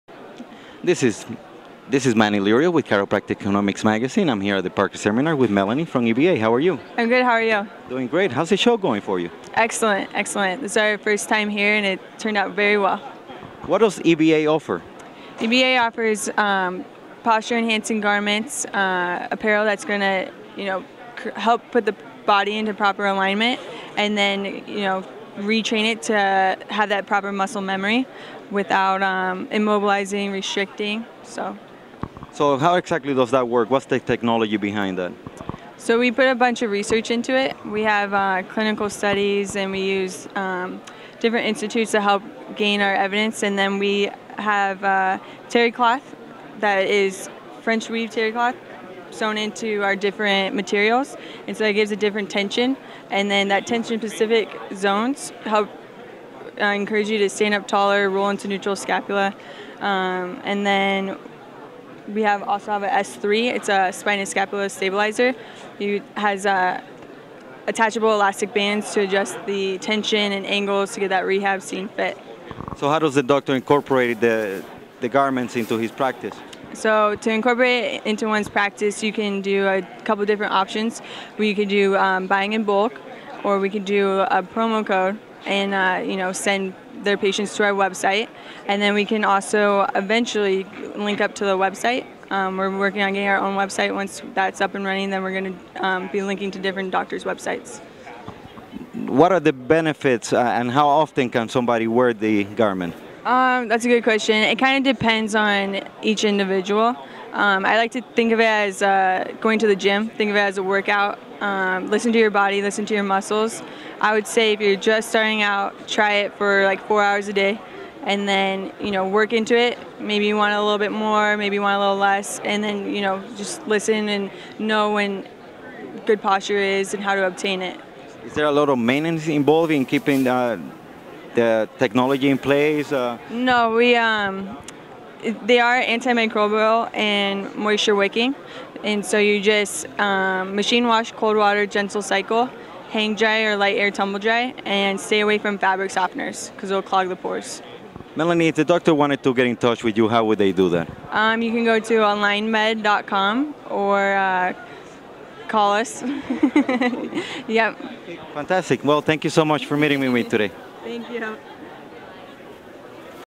Alignmed Inc. - Parker Seminars Las Vegas 2012
Manuel Lirio, VP Web Operations and Audience Development for Chiropractic Economics, interviews Melanie Grosvenor, Special Teams Representative for Alignmed Inc., about the company's Evidence-based Apparel Posture Shirts.